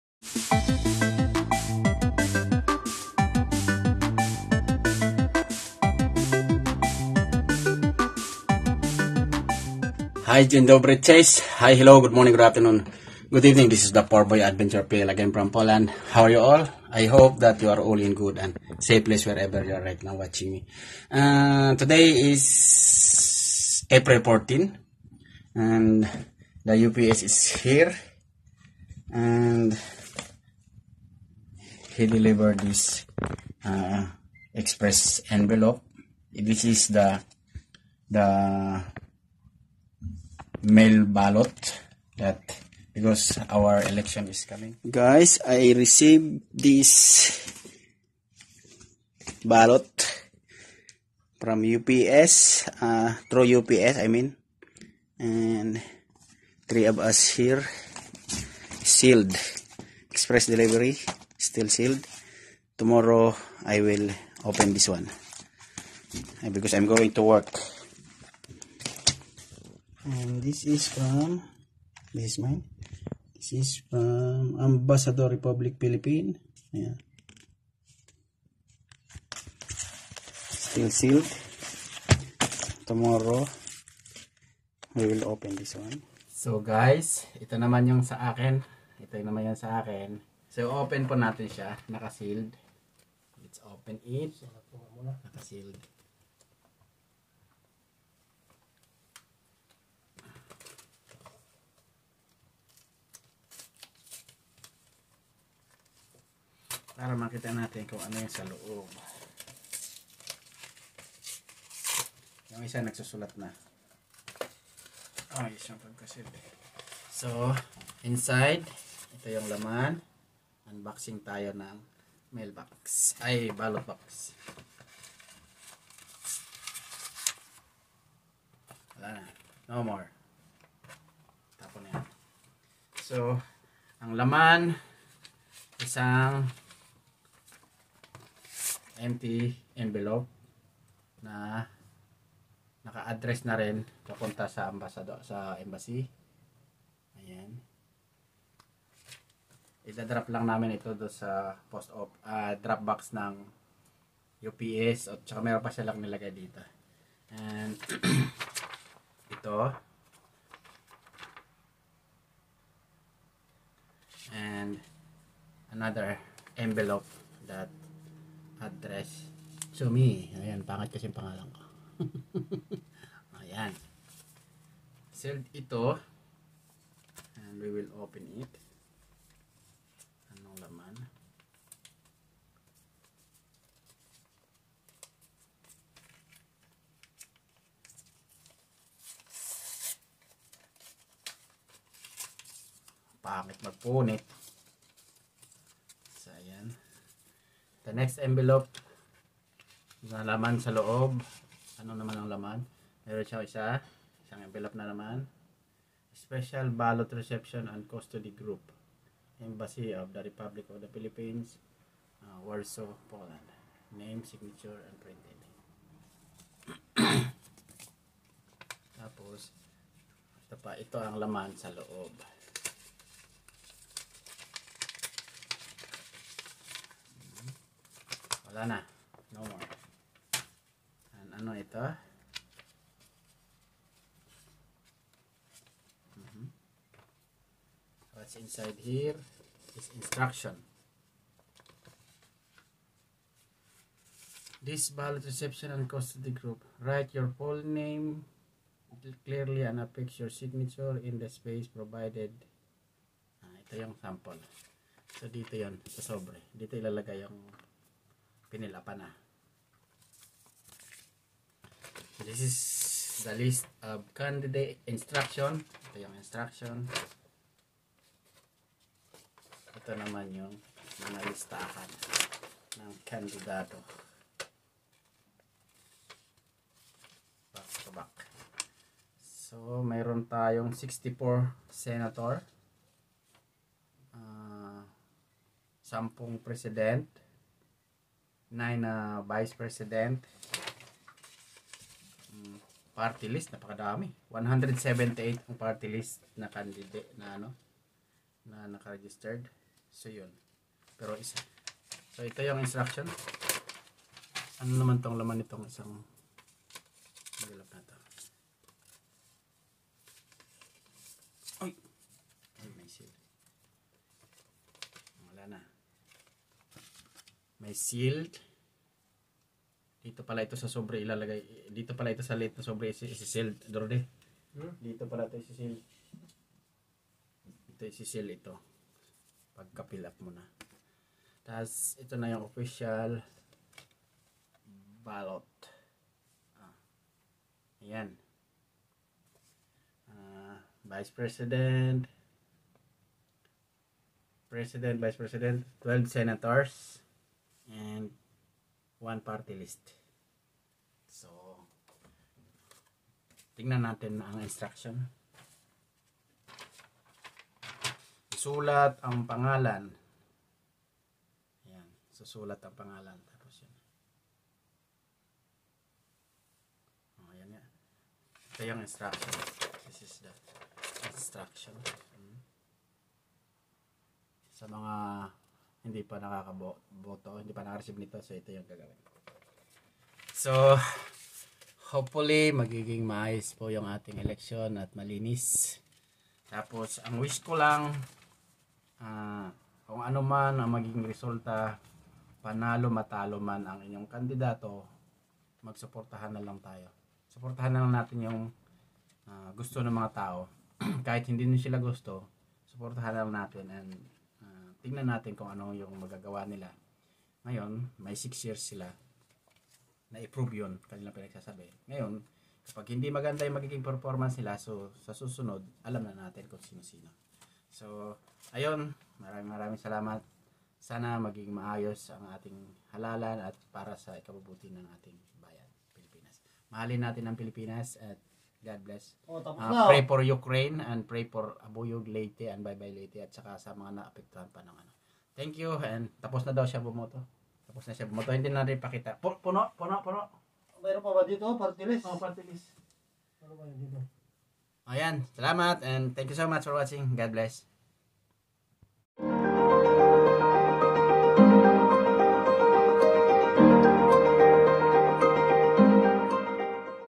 My Ballot for National Election/overseas Mail Voting
Me and my Housemates Ballots arrived, Because we are using mail Voting as Overseas Voters